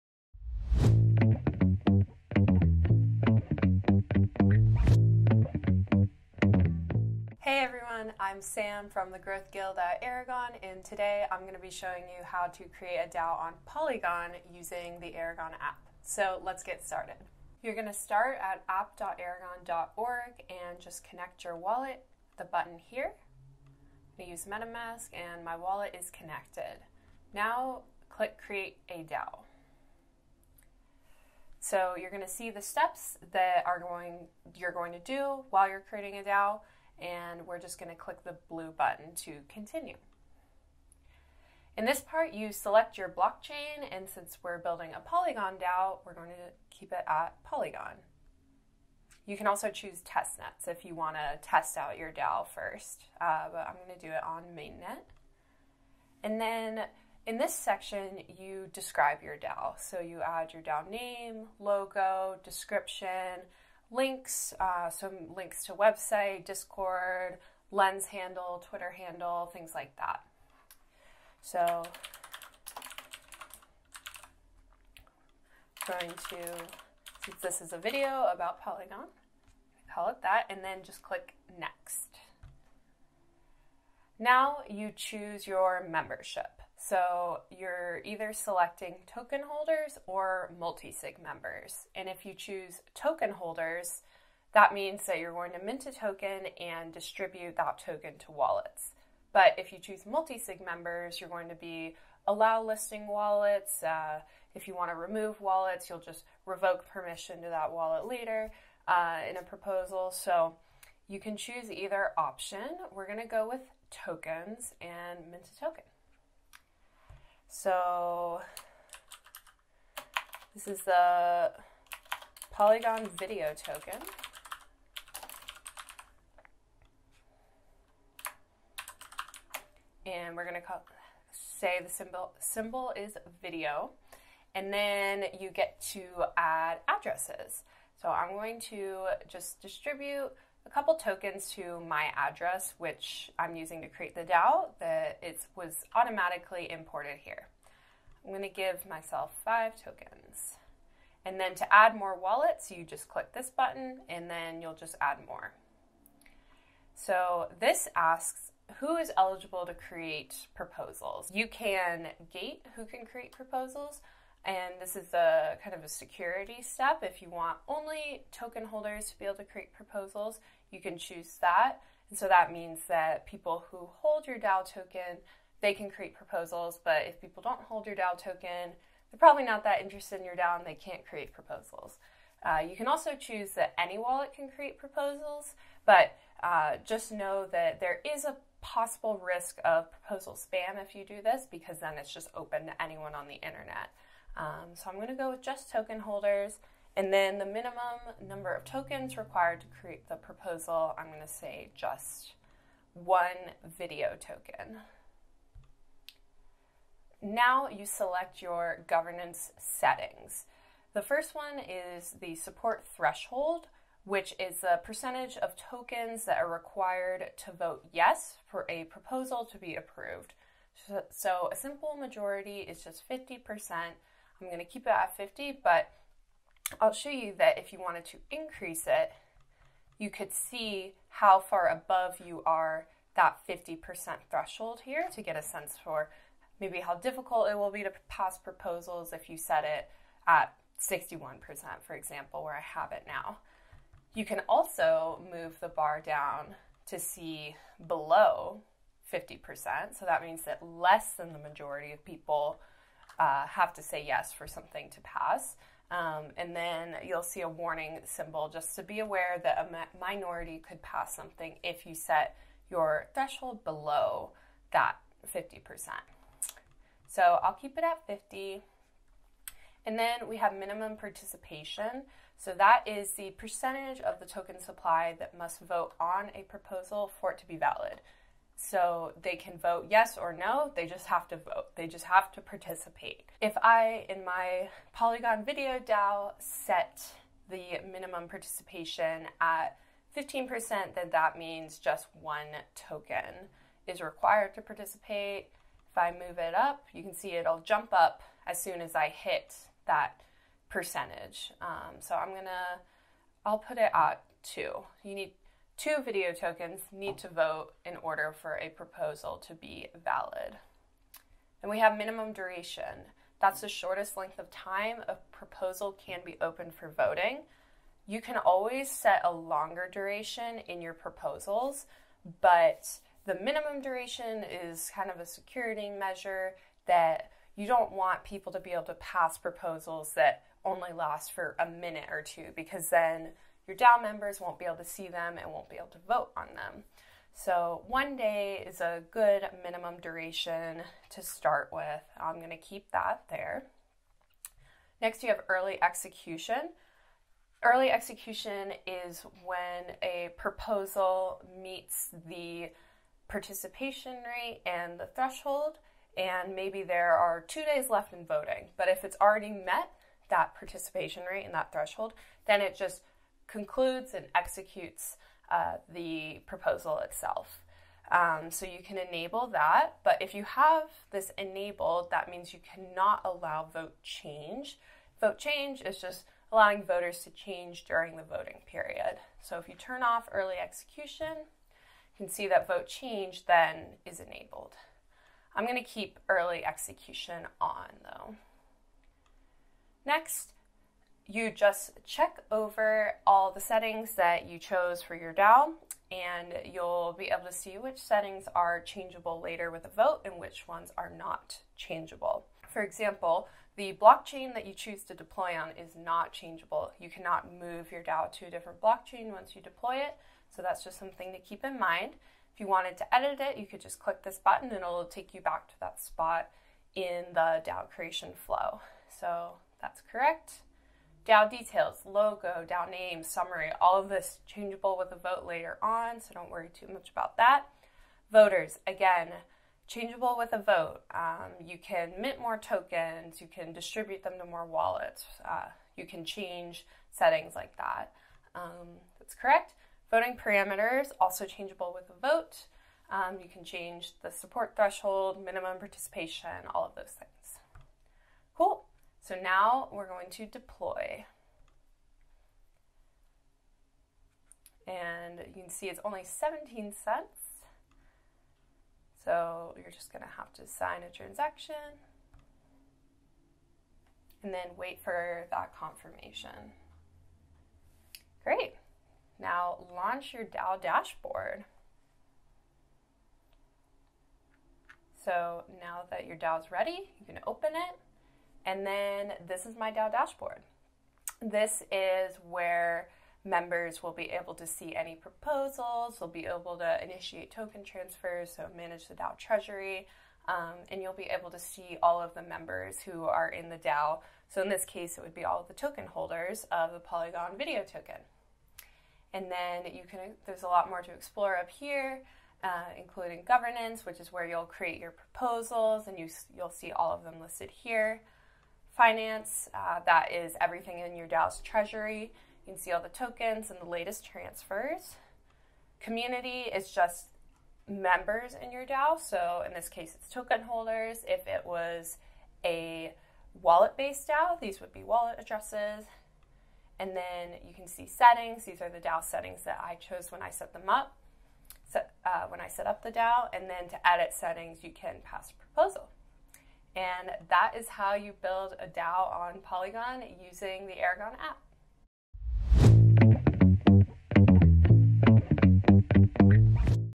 How to Create a DAO on Polygon with the Aragon App
Choosing to create a DAO on Polygon is a great way to save money and time for your members, because Polygon has lower gas costs and faster transactions than Ethereum. 💜

It only takes a few minutes to launch your DAO on Polygon and start working toward your mission today!‍ 🚀

In this guide, we’ll walk through the steps to create a DAO on Polygon using the Aragon App! If you need more information, you can read the written guide